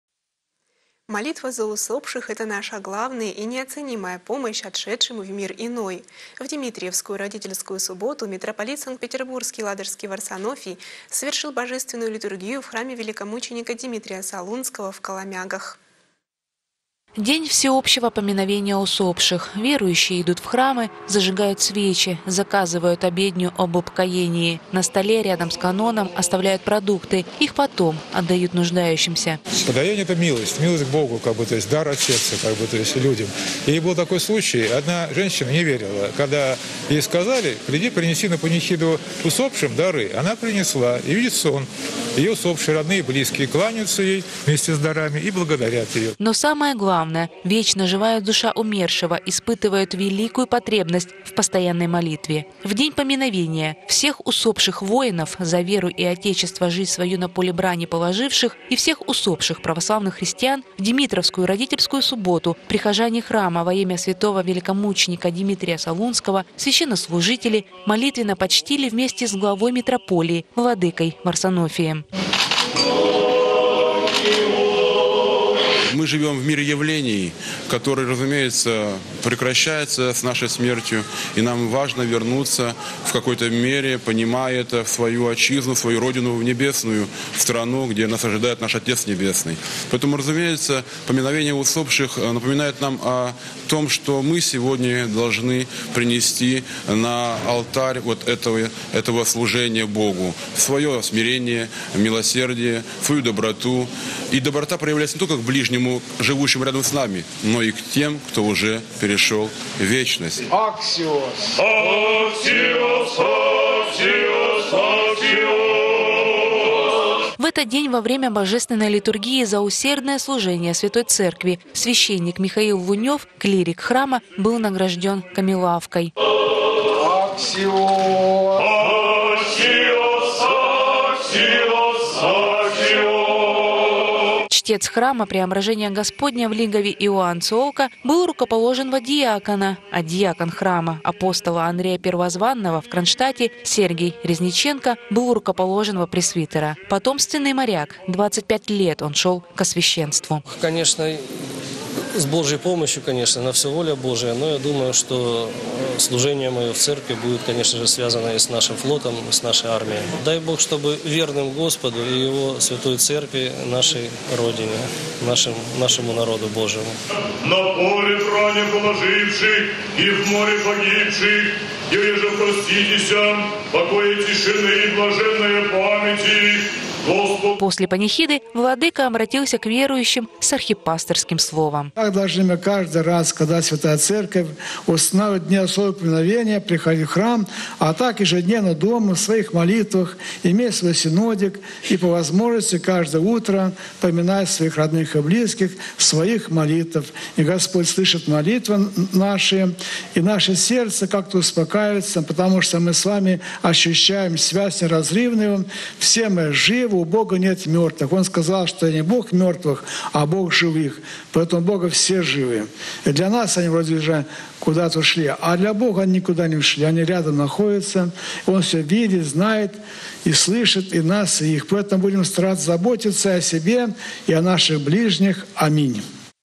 В родительскую субботу храм влмч Димитрия Солунского в Коломягах посетил глава митрополии
Молитва за усопших – это наша главная и неоценимая помощь отшедшим в мир иной. В Димитриевскую родительскую субботу митрополит Санкт-Петербургский и Ладожский, Варсонофий совершил Божественную литургию в храме великомученника Димитрия Солунского в Коломягах.